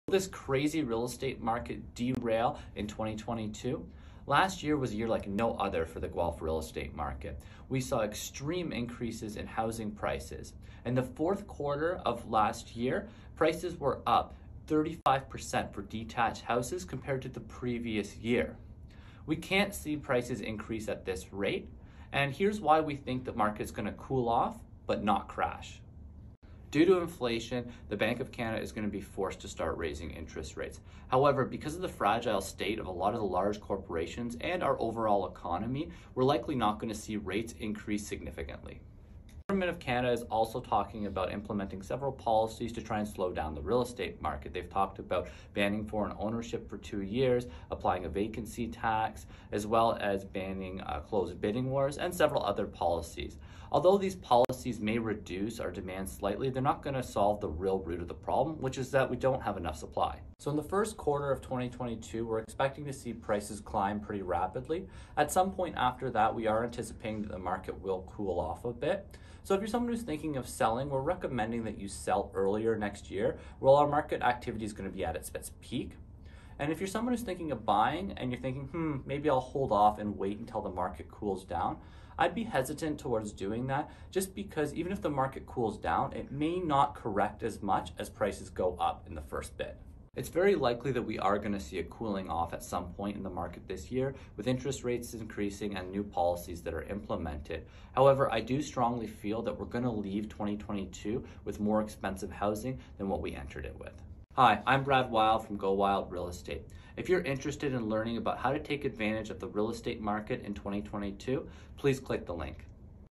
Will The Run-Away Real Estate Market De-Rail In 2022?
Last year was a year like no other in Real Estate. We experienced unprecedented housing price increases in Guelph. The 4th quarter compared to the same period last year saw an average price increase of 35% for a detached home in Guelph. The market can not continue to rise at the current rate.

Here’s why we think it’s going to level off and not crash.
Due to inflation, the Bank of Canada will start to raise interest rates. However, because of the fragility of several large corporations and our economy, we likely won’t see significant increases.

The government is talking about several new policies such as banning foreign ownership for 2 years, taxing vacant homes, banning closed bidding wars, amongst other additional policies.   Although some of these measures may slightly reduce demand, it is not going to be enough to solve the root problem which is supply.

In the first quarter of 2022, we will probably see housing prices continue to climb rapidly.   At some point, after that, we believe the market will level off.   So if you are thinking of selling next,  early in the year will be better.  If you thinking of buying you may want to buy at the beginning of the year before prices increase.

There will definitely be a cooling of the real estate market this year as interest rates increase and some policies are implemented,  but we believe we will leave 2022 with more expensive houses than we entered.